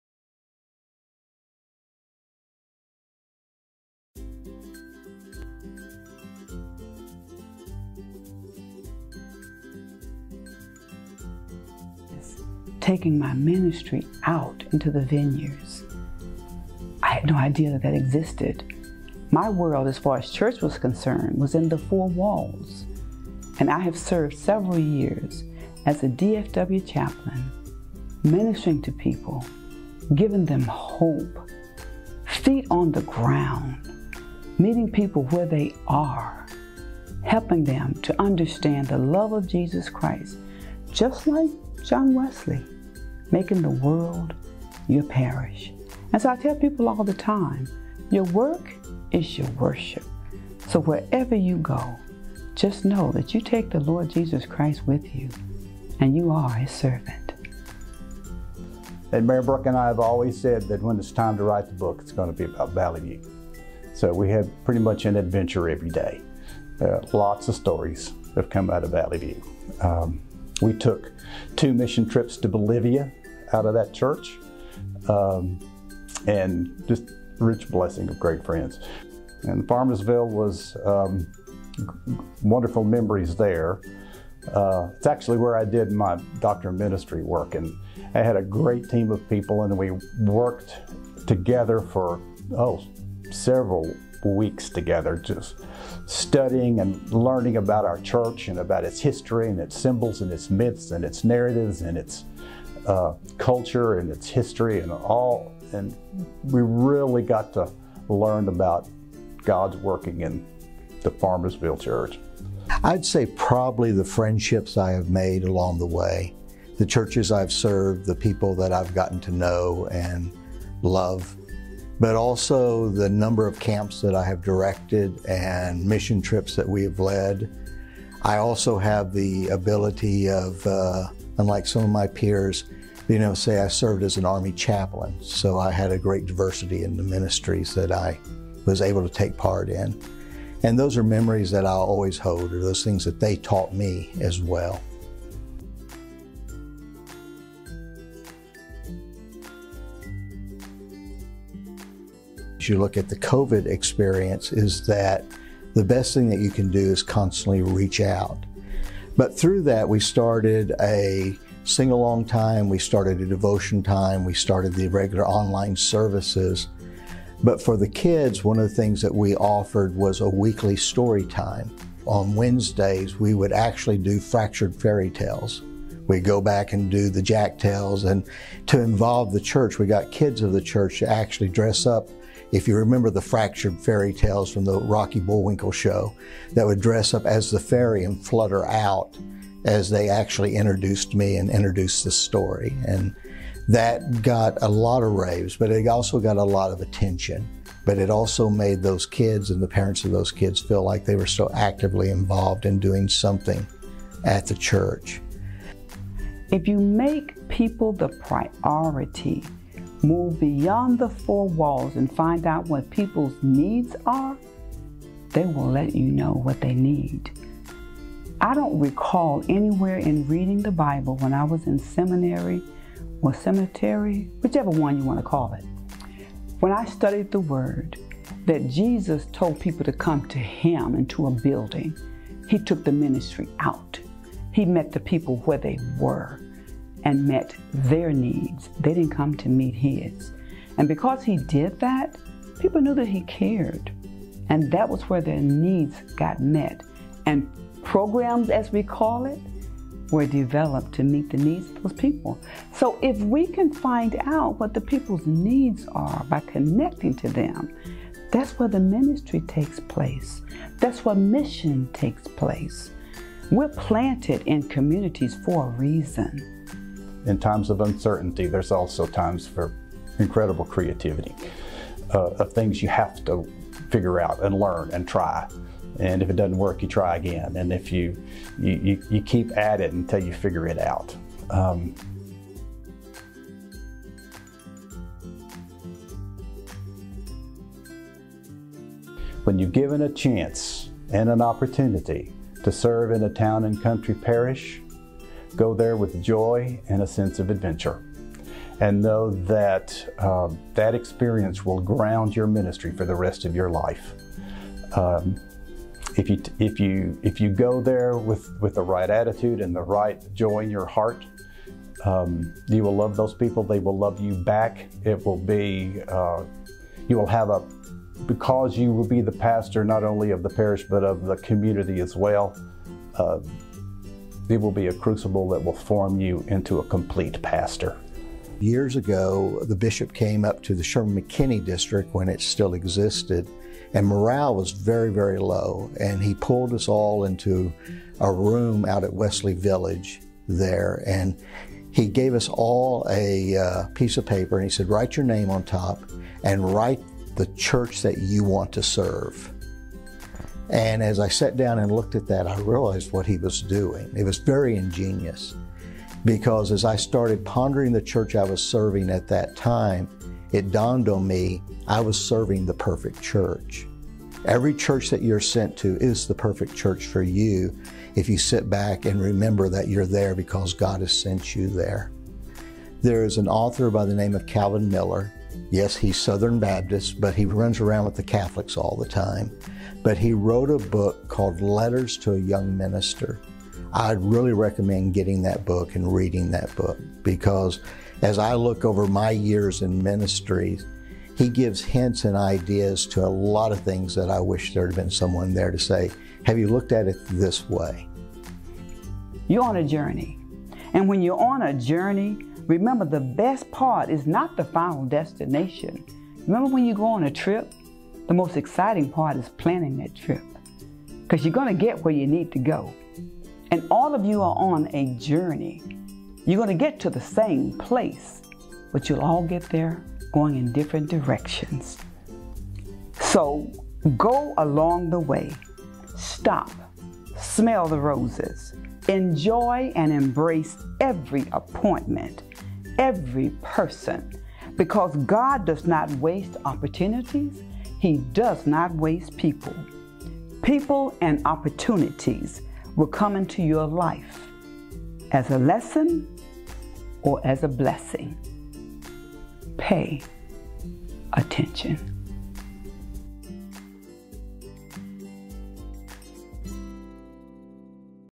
2021 North Texas Conference retirees (2 of 3)
Listen to these 2021 retirees share what they cherish most about The United Methodist Church and their hopes for the North Texas Conference: Rev. Veronica G. Davidson, Rev. Dr. Victor Casad and Rev. Tommy Ray Medley.